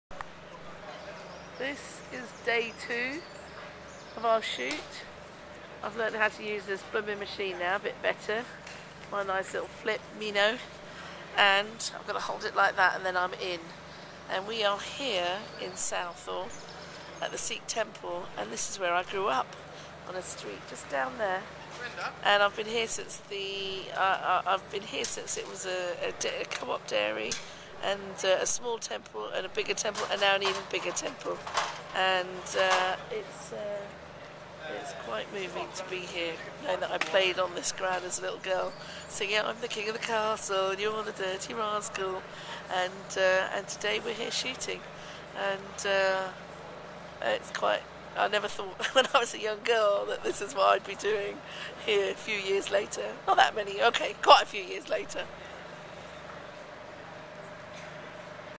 GurinderCam: Episode 2 - filming at temple for It's a Wonderful Afterlife
Citroen in view is a real car!
It's a Wonderful Afterlife - Making of the Movie